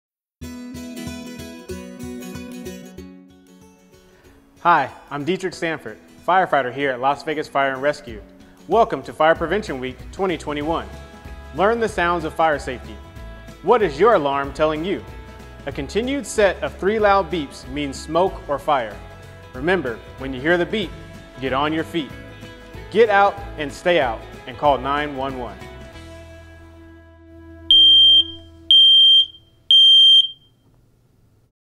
Fire Prevention Week 2021 - Smoke alarm PSA
Do you know the sounds of Fire Safety? Learn the Sounds of Fire Safety this week during Fire Prevention Week 2021.